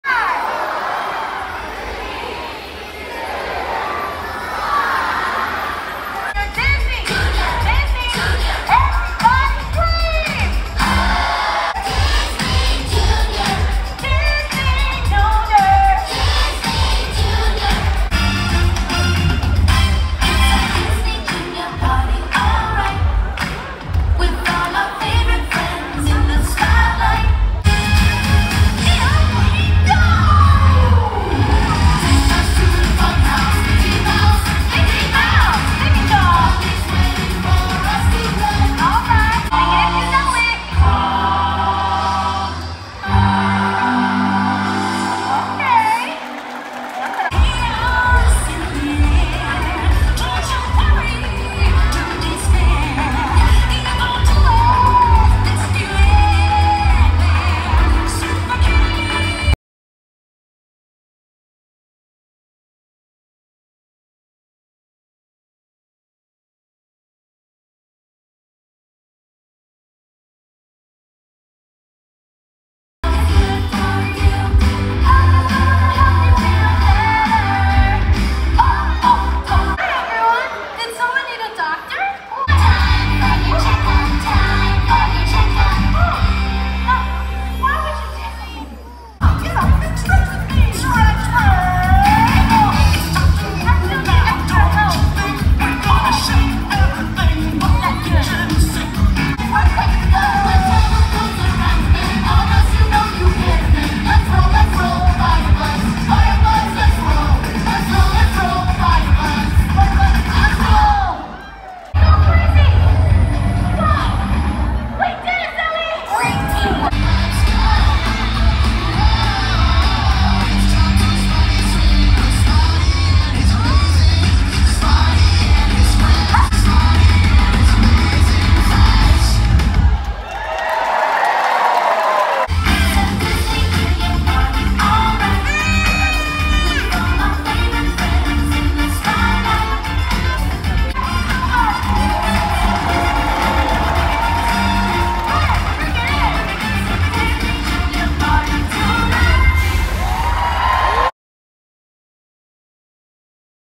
Disney Junior LIVE ON TOUR Costume Palooza! September 10, 2023
“Disney Jr. Live: Costume Palooza” is the first show of its kind for pre-school aged kids. Those as young as two are sure to enjoy the singing, dancing and web-slinging fun!